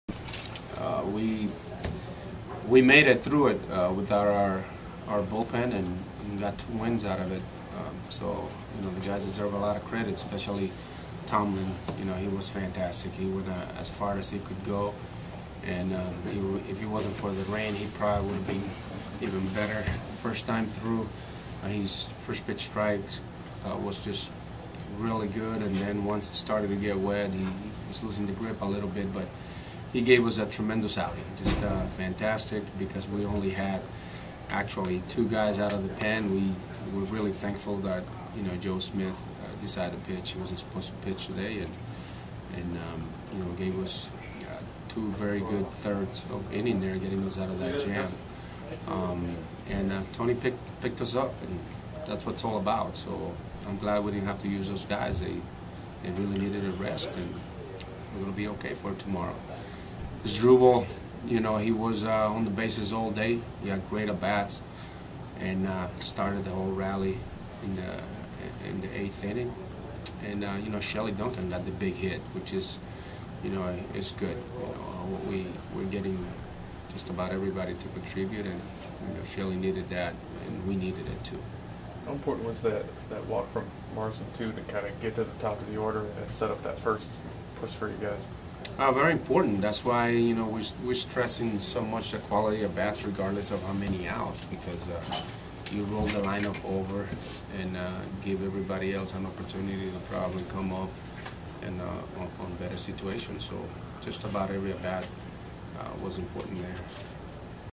May 7, 2012: Manny Acta following 3-2 win over White Sox
On Monday, May 7, the Cleveland Indians completed a sweep over the Chicago White Sox winning the second game of a day-night doubleheader 3-2 at Progressive Field. After the game Tribe skipper Manny Acta talked about the two victories, the use of his short-handed bullpen, continued play and the inclement weather that invaded downtown Cleveland for separate rain delays.